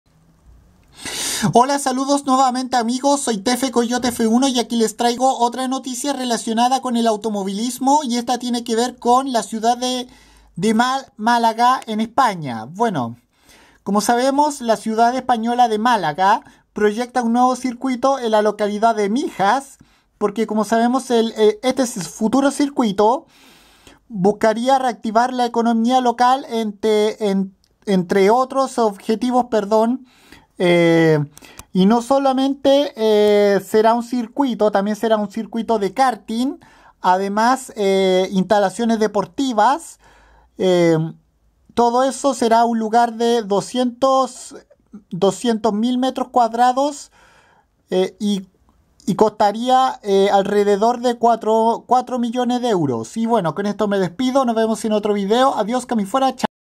Málaga se proyecta una nueva pista de carreras en la localidad de Mijas
El nuevo circuito de carreras de Málaga tendría instalaciones deportivas y una pista de karts y buscará reactivar la economía local entre otros objetivos